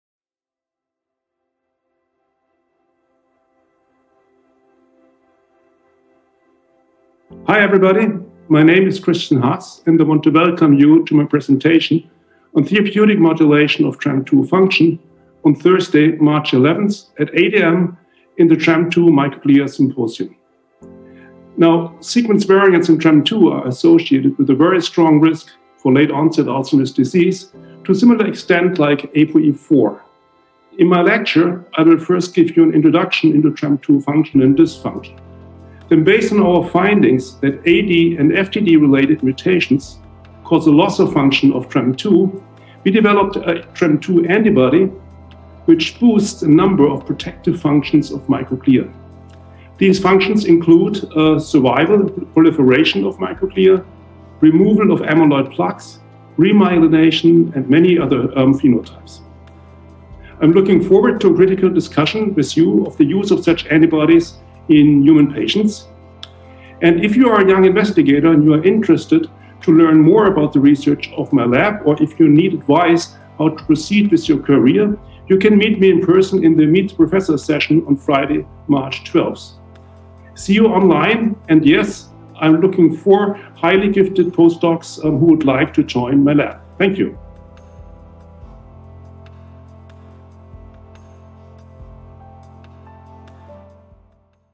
AD/PD™ Speakers - Prof. Christian Haass
“See you online! And yes – I am looking for highly gifted post-docs who would like to join my lab!”

Hear from Prof. Christian Haass why ADPD2021 will be all about collaboration. Many networking opportunities await you in March – this is your conference!